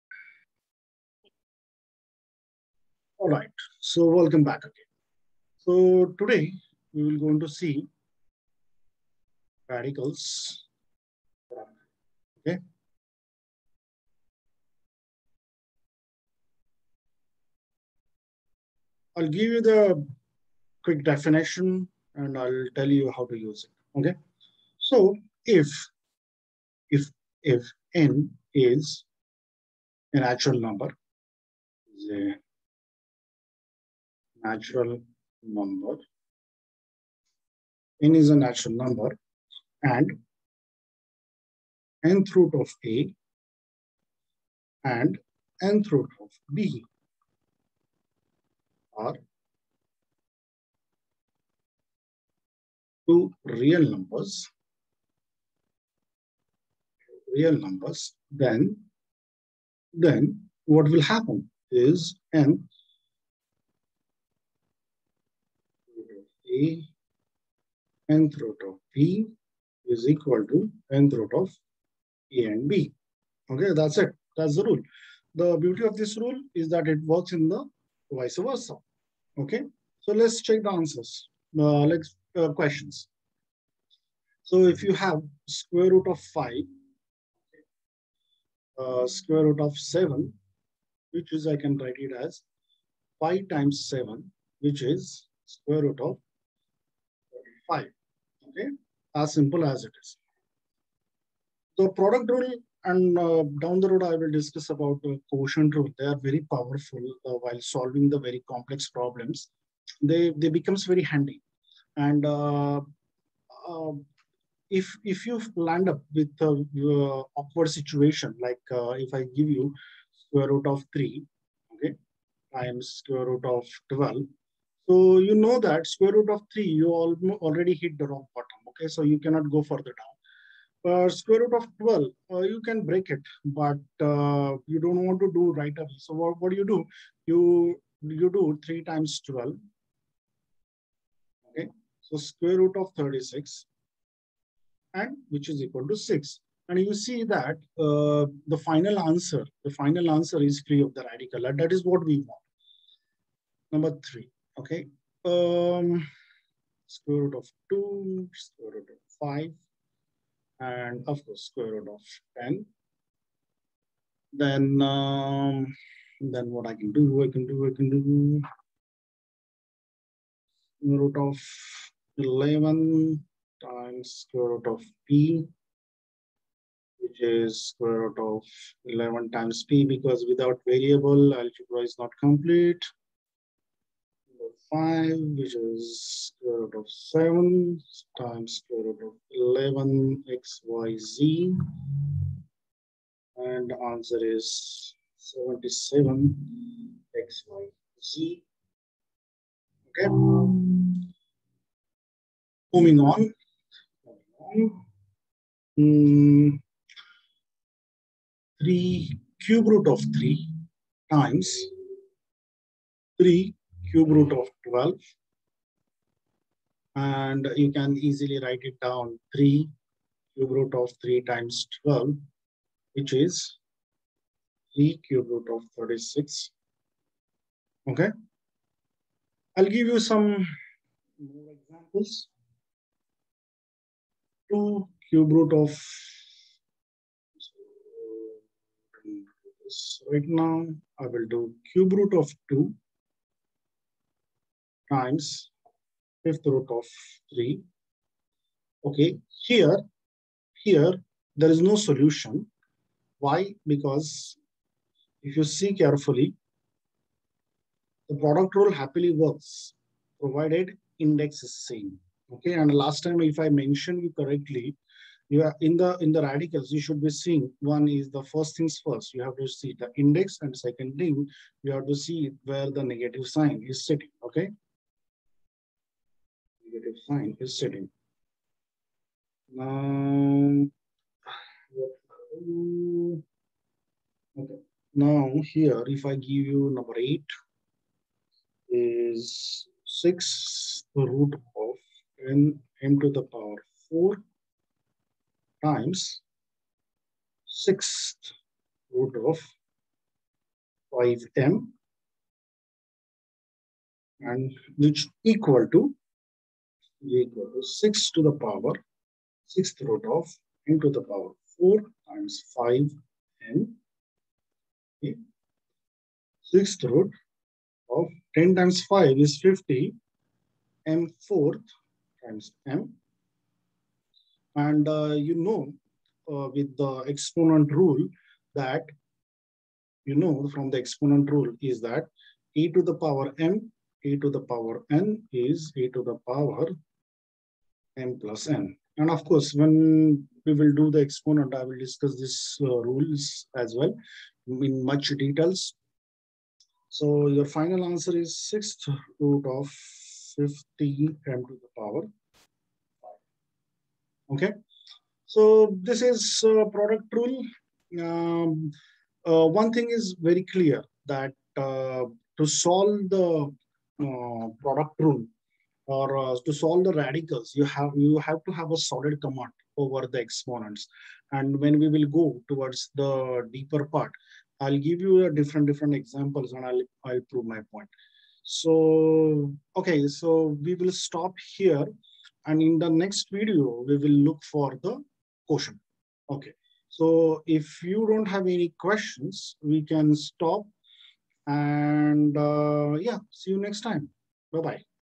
Radicals, Product Rule
In this lesson, we are going to study Product rule in Radicals.